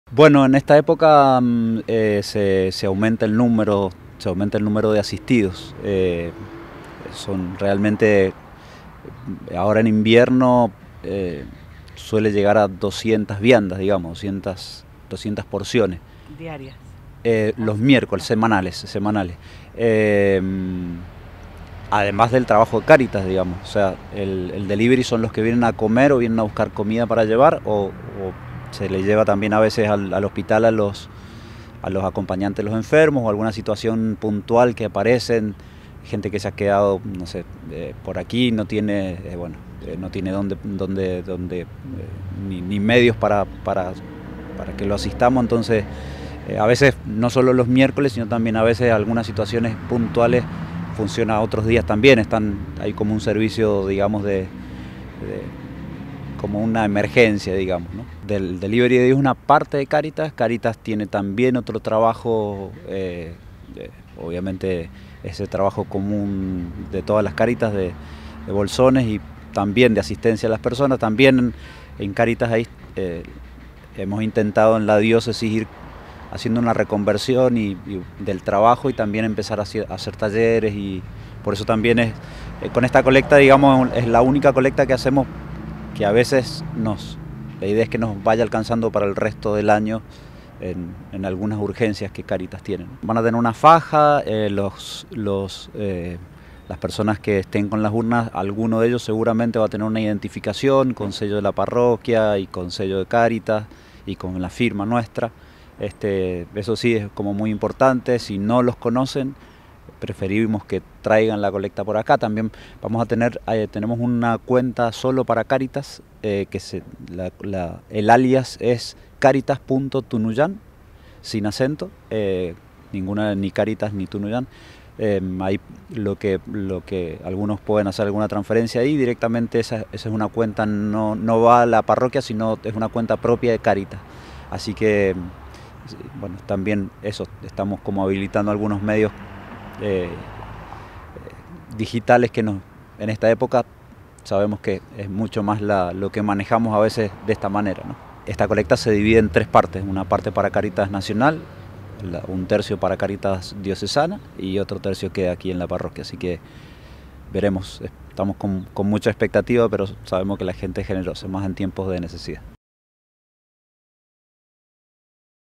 Tunuyán - ¿Cómo trabaja Cáritas?
El Cura Párroco de la Iglesia Nuestra Señora del Carmen, Pablo López nos comentó sobre la labor que llevan a cabo durante todo el año con la gente de Cáritas y la asistencia a familias que lo necesitan.